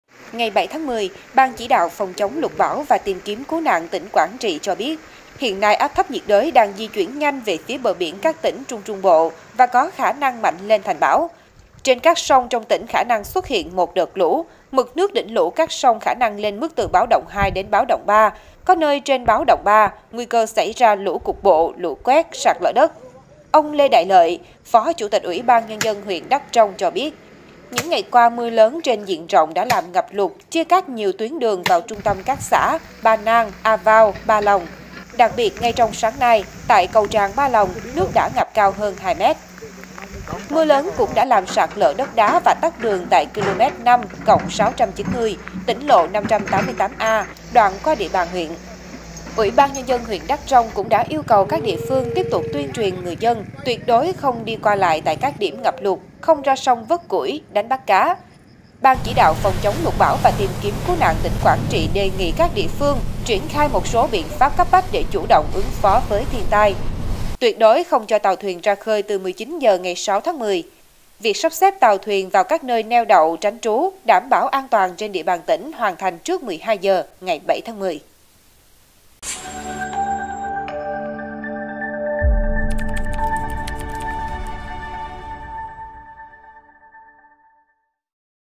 Mưa lớn gây chia cắt nhiều khu vực miền núi tỉnh Quảng Trị
Ngày 7-10, Ban Chỉ đạo phòng chống lụt bão và tìm kiếm cứu nạn tỉnh Quảng Trị cho biết, hiện nay áp thấp nhiệt đới đang di chuyển nhanh về phía bờ biển các tỉnh Trung Trung Bộ và có khả năng mạnh lên thành bão.